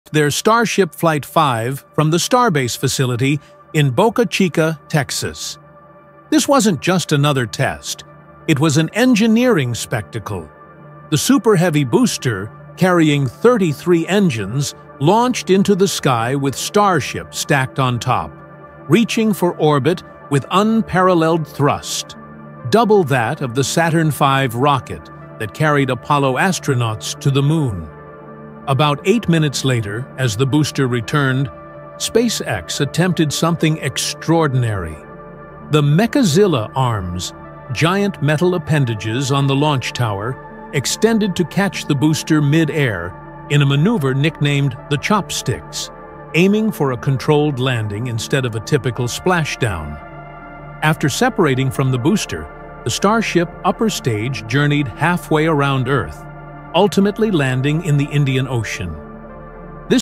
Elon Musk's New Rocket Launch: SpaceX Starship 2024
Join us on a journey to SpaceX’s latest groundbreaking feat! 🚀 On October 13, 2024, Elon Musk’s team launched Starship Flight 5, taking rocket engineering to new heights with the incredible 'Mechazilla' catch—using giant robotic arms to capture a returning booster! This launch marks a crucial step toward reusable rockets and future Mars missions, supporting NASA’s Artemis project and Elon Musk’s vision of a multiplanetary future. 🌍➡️🚀➡️🌌 Watch as we dive into how SpaceX is reshaping space travel!